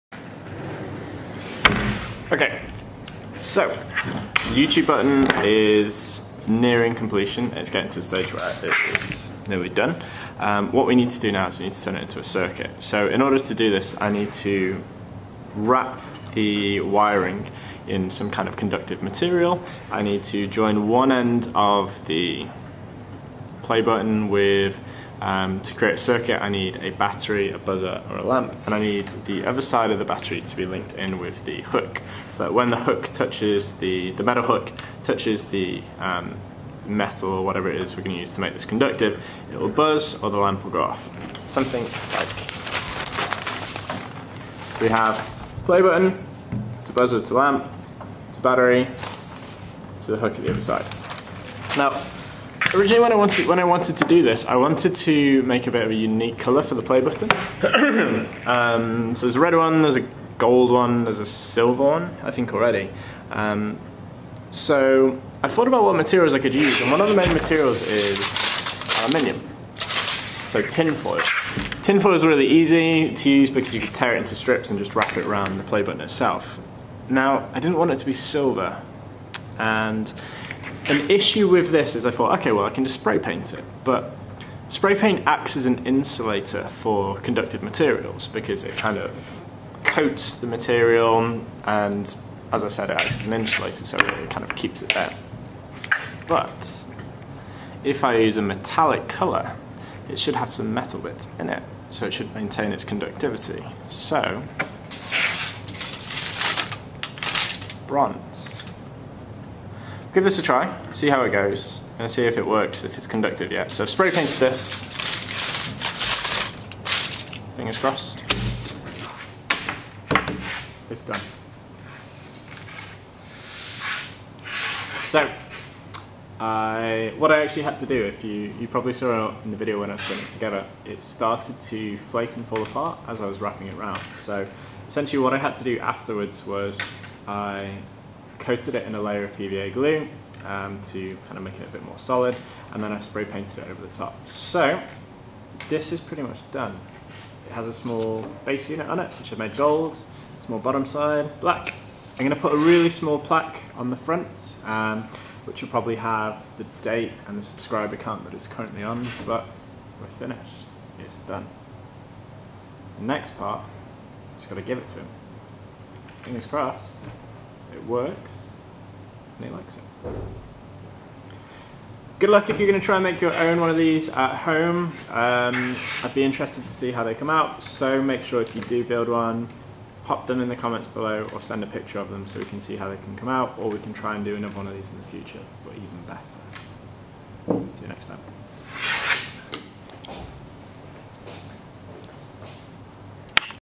Completing the Play Button [Part 3]
In this video we complete our custom Play button! Can we add some creativity whilst maintaining conductivity?

Thank-you for following along during the creation of this, why not try your own and share the results?